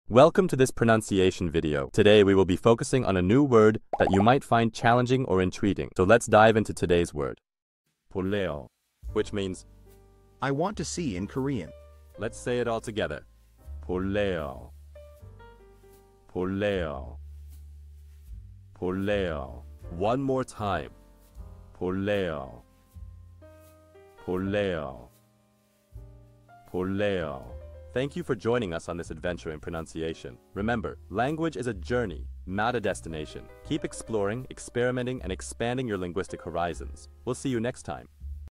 How to pronounce bolraeyo | 볼래요 (I want to see in Korean)
Master the Pronunciation of 'bolraeyo | 볼래요' - which means : I want to see in Korean 📝 with @PronunciationKorean 🎓🗣️ - [Your Guide to Korean]

Learn to pronounce 'bolraeyo' perfectly in Korean with our easy and engaging tutorial by @PronunciationKorean! 📖📽️ In this comprehensive video, we provide you with native speaker examples 🗣️👂 and phonetic breakdowns 🎵 to ensure you get the pronunciation just right.